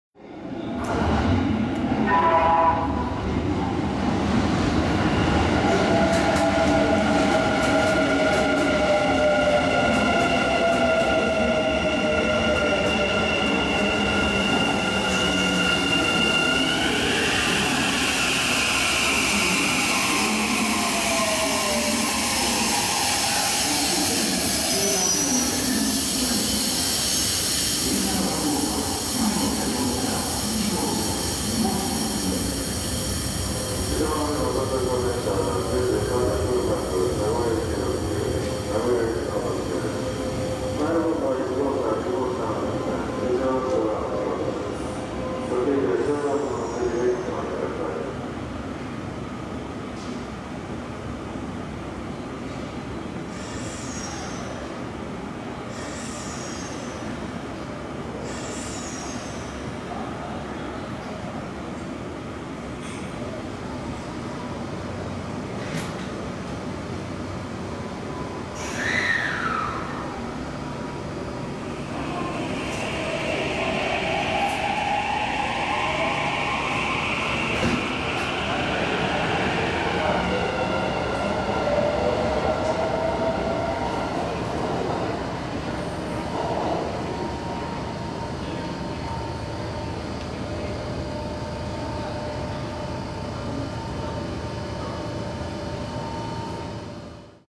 近鉄50000系 送り込み回送 大阪難波駅 到着～発車
撮影日2015/03/22
高安からの回送列車が大阪難波駅に到着して発車するところです